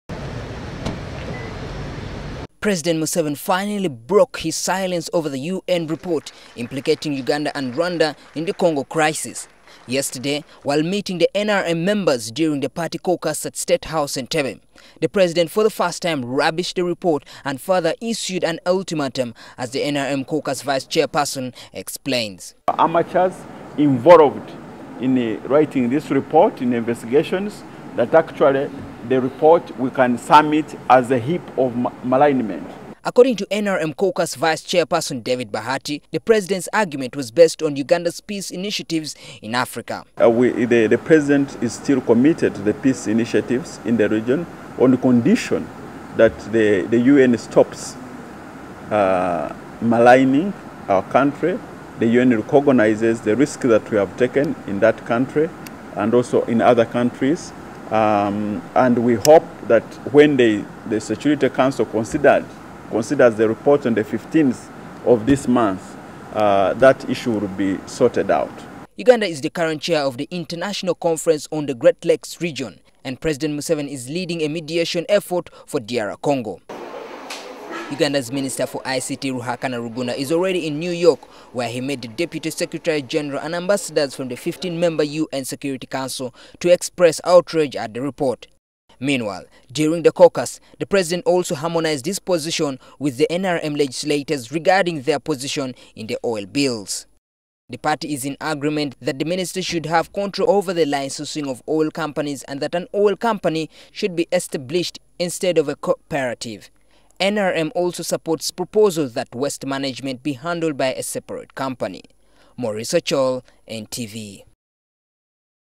THE PRESIDENT'S ULTIMATUM: MUSEVENI WANTS UN REPORT DROPPED IN 10 DAYS
President Yoweri Museveni has given the United Nations an ultimatum of up to the 15th of this month to withdraw its report on Uganda's alleged involvement in the Eastern Congo region.
The president made the announcement while addressing NRM members at State House, Entebbe.
And as Maurice Ochol reports, the NRM caucus finally harmonized their position on the Oil bills.